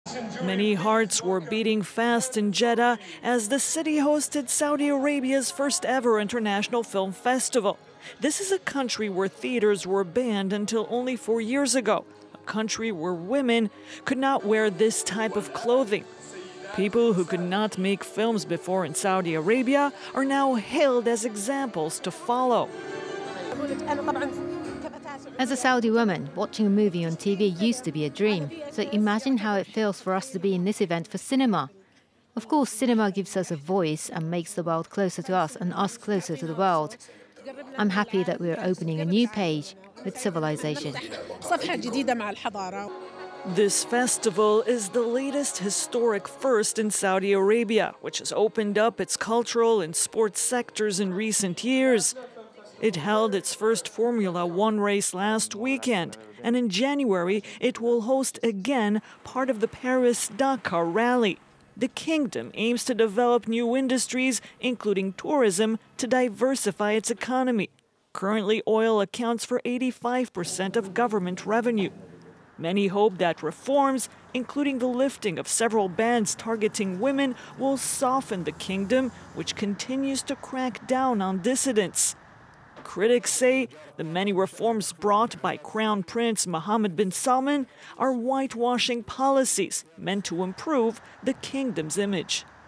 From cinema ban to film festival: Saudi rolls out red carpet • FRANCE 24 English
Less than four years after lifting a ban on cinema, SaudiArabia rolled out the red carpet Monday in the Red Sea city of Jeddah for celebrities descending on the kingdom's first major film festival.